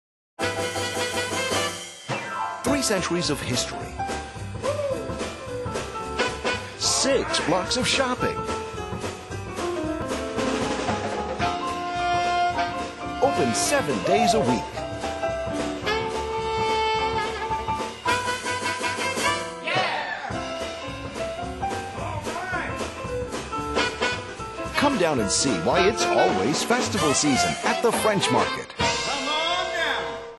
French Market in New Orleans
This is the new spot for the French Market in New Orleans. We spent one full day shooting. WWL-TV Production Crew - Director/Editor: Kirk Jacobs, Executive Producer: Kell Remkes, Photographer: Paul Corcoran, Grips: John Carambat, Will Bezubic.